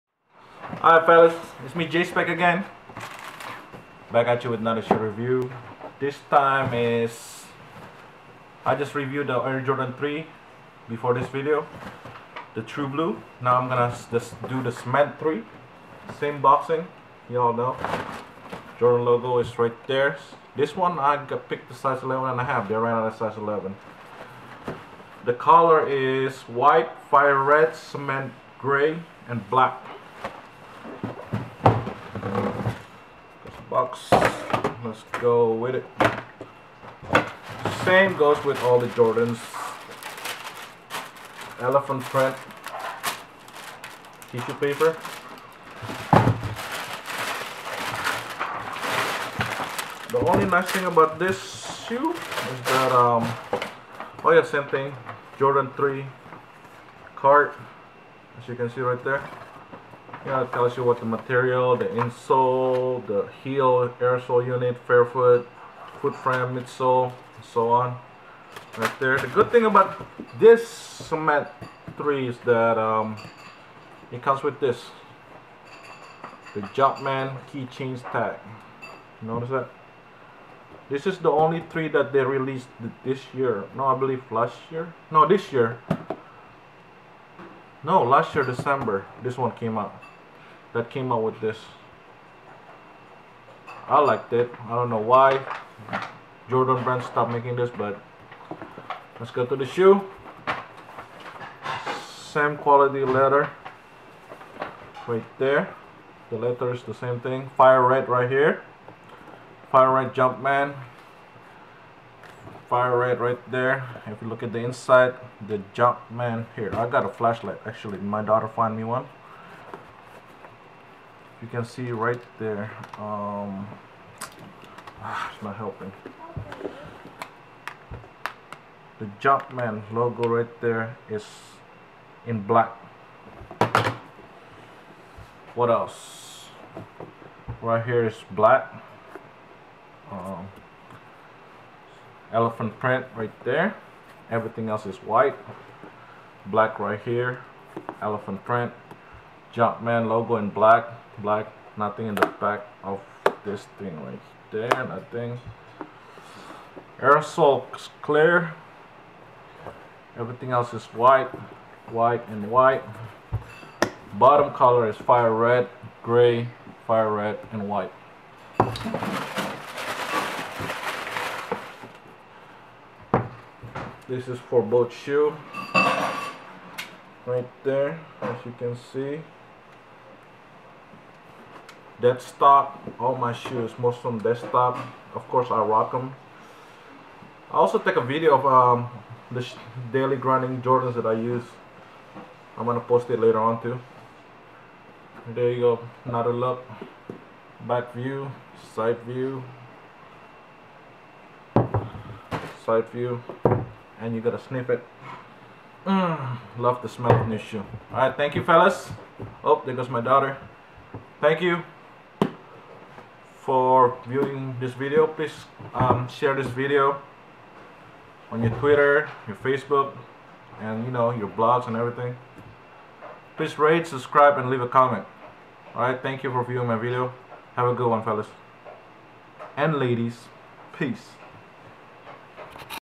2010 Air Jordan III 3 Retro "White Cement" Sneaker Review In Full HD
Enjoy this video on shoe review for Air Jordan CEMENT 3 RETRO.